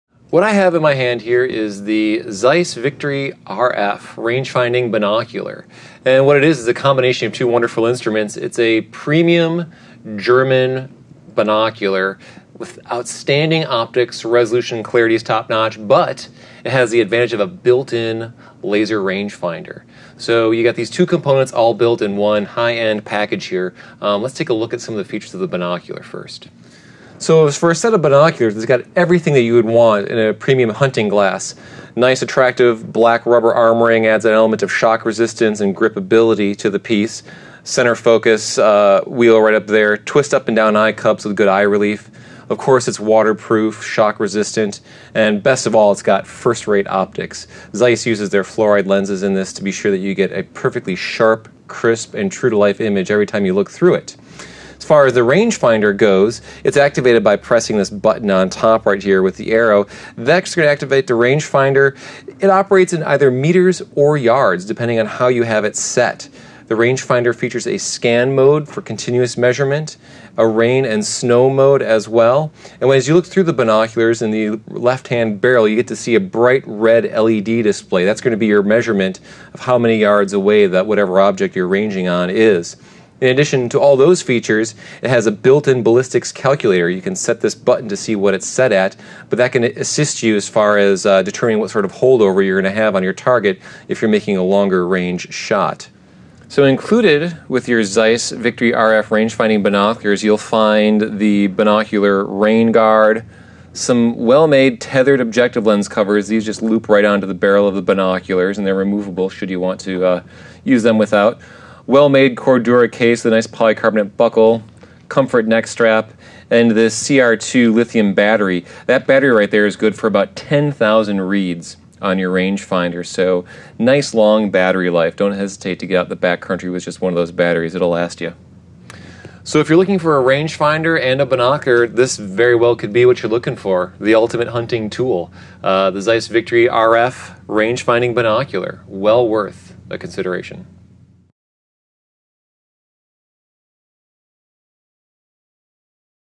Zeiss Victory RF Rangefinding Binoculars
Take the unbelievable brightness of a Zeiss Victory binocular, add a rangefinder with state-of-the-art features, and you have the award-winning Zeiss Victory RF rangefinder binocular. Honored with design awards and the coveted Outdoor Life Optics Test Editor's Choice award, the Victory RF will delight you with its exceptional performance.